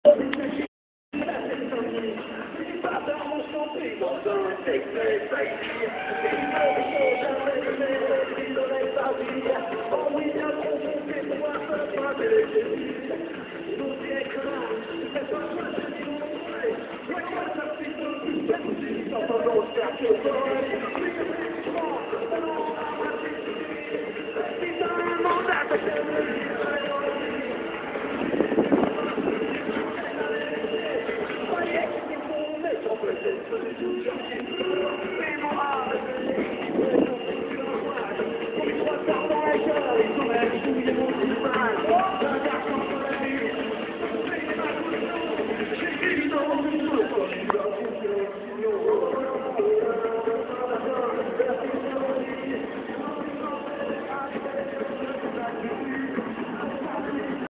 Friture express
Incendie du chalutier du musée maritime de La Rochelle avec en fond le concert des franco.